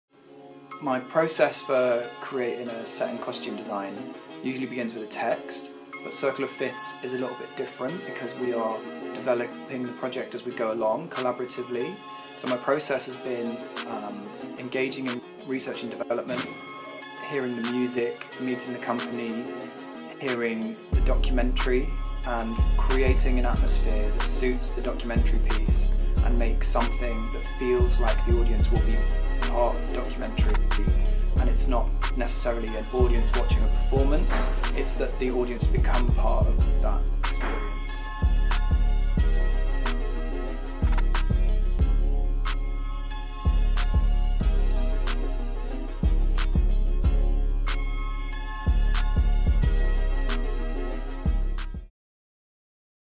Circle of Fifths | Jacob Hughes on designing the stage
"Creating an atmosphere that suits the documentary piece and something that feels like the audience are part of the documentary."